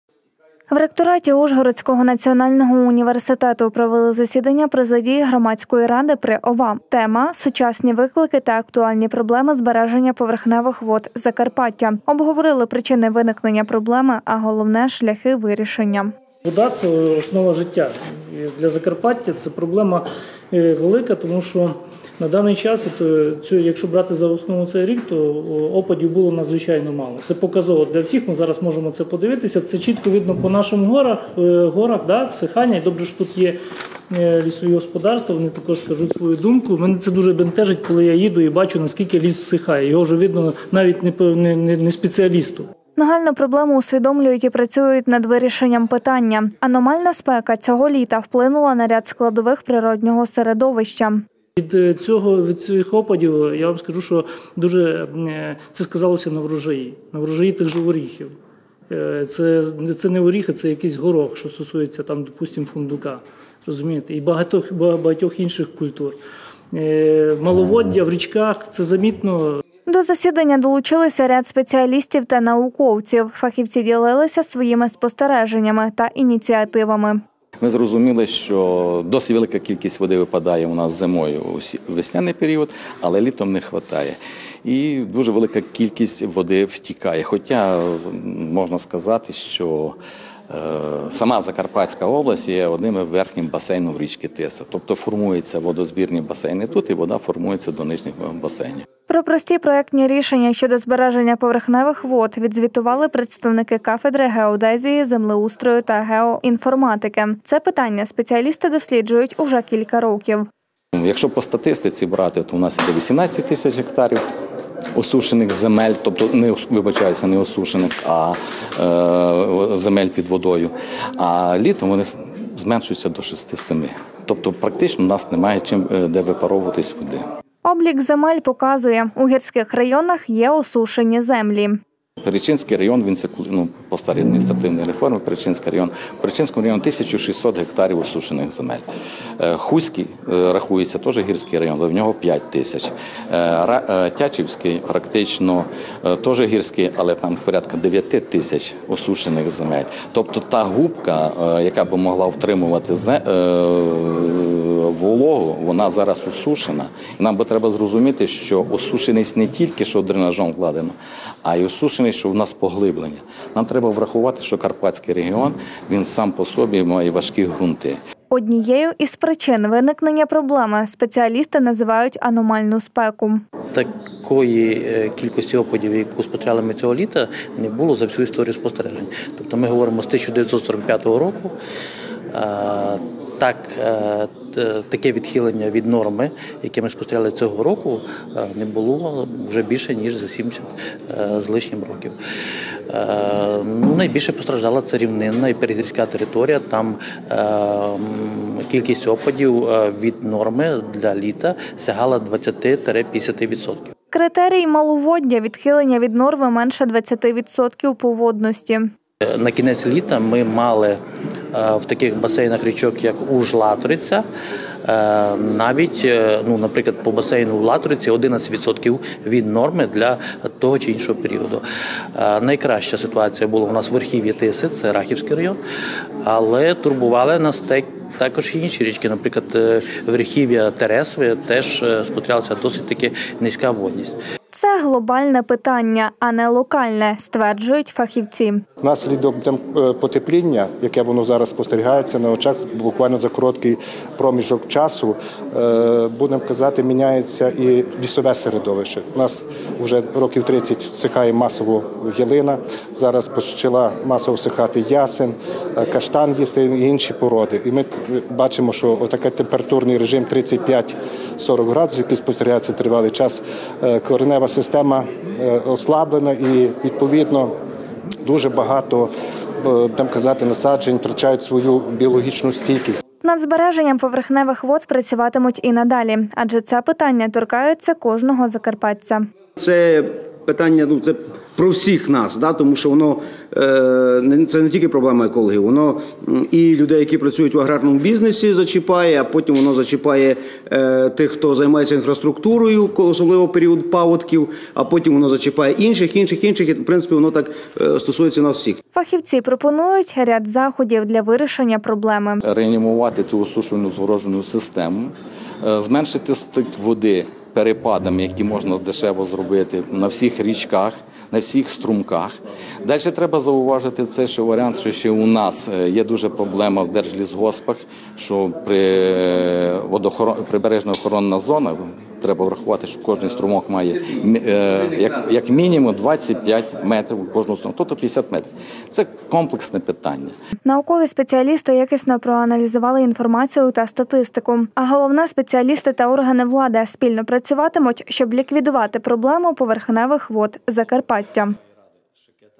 Засідання президії. Проблеми збереження поверхневих вод Закарпаття.
«Сучасні виклики та актуальні проблеми збереження поверхневих вод Закарпаття» - в великій залі ректорату Ужгородського національного університету провели розширене засідання Президії Громадської ради при Закарпатській обласній військовій адміністрації.